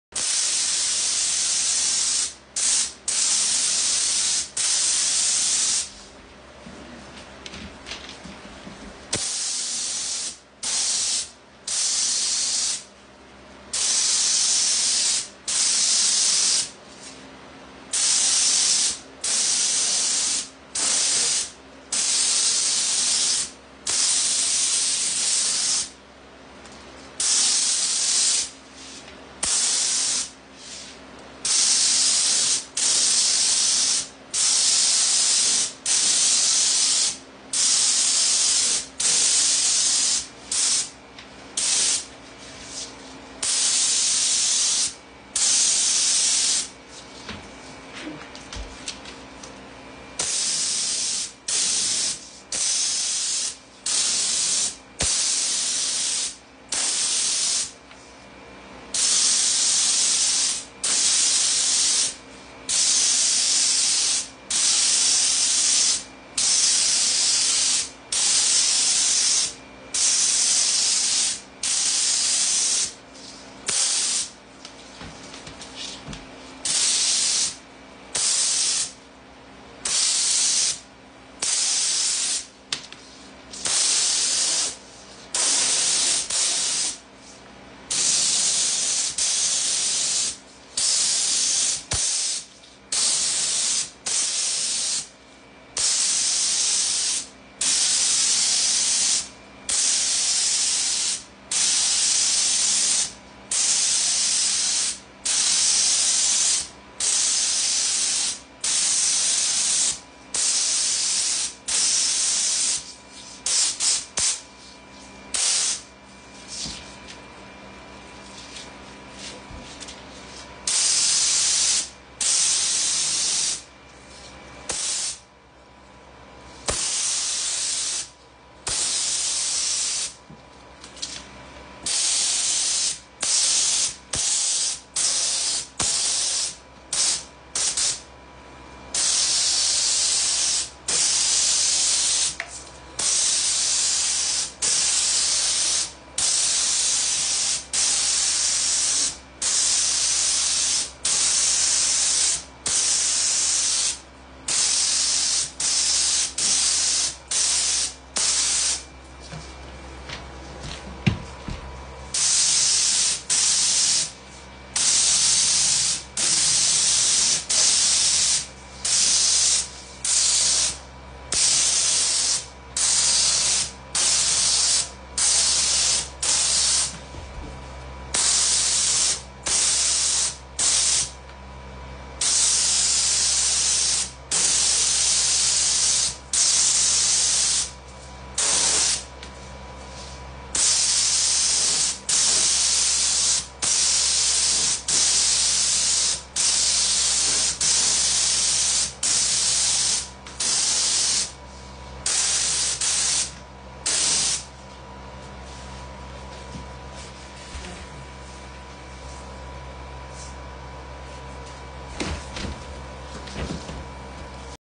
How to spray Kitchen Cabinets
Applying satin lacquer to the island in super white. Using a HVLP gun and spray system the cabinet panels come out smooth as glass.